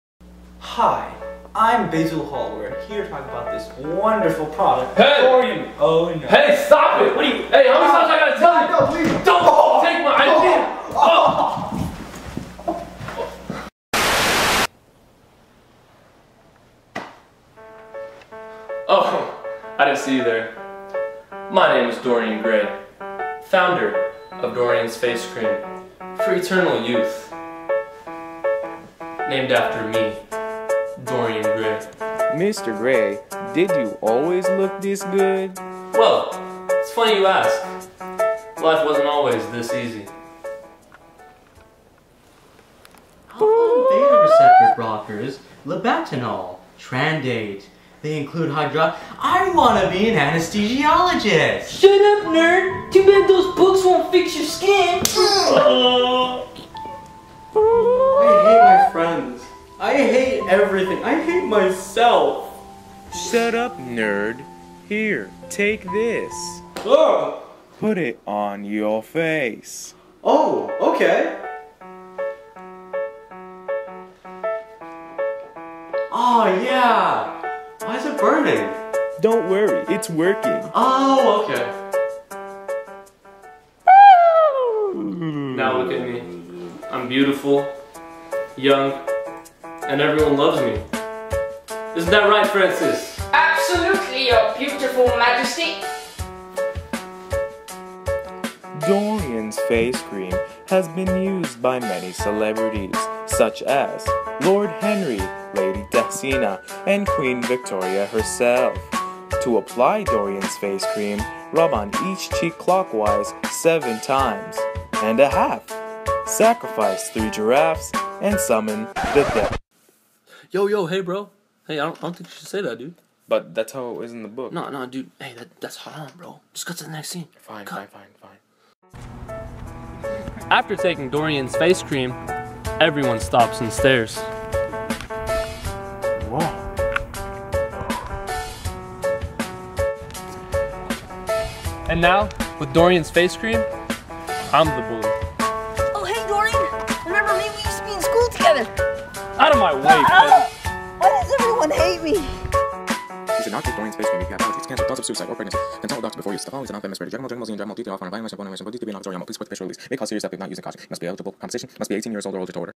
[2015-16] Dorian's Face Cream
Presented by Yaman Elsharydah, Abdullah Orfaly, Ibrahim Boutaleb, Mustafa Ahmad, Abdurahman Fridi.
To be presented for the Dorian Gray Showcase.